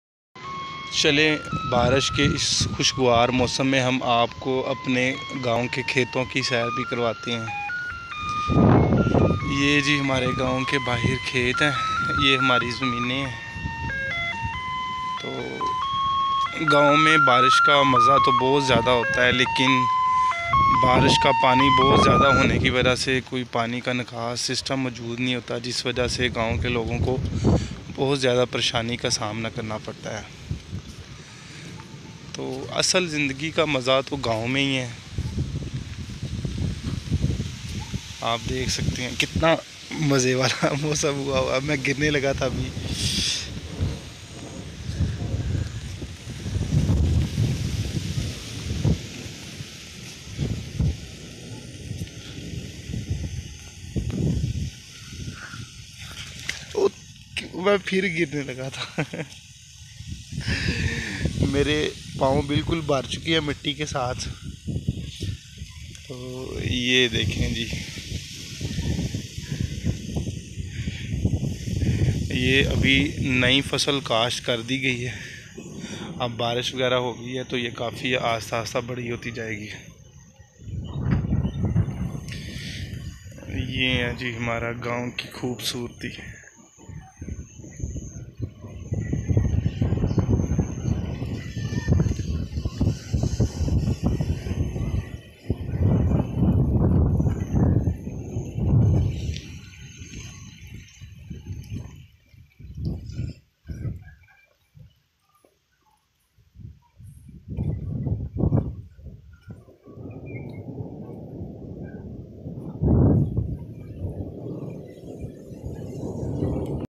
Rain in our Village. (Rainy day 🌹)
It is beautiful to swim during rain fall as the water 💦 splashes your face and give you great relief 😜🤠
Rain days give us the serendipity to spend time with ourselves And our families..😘🖕🖕☹️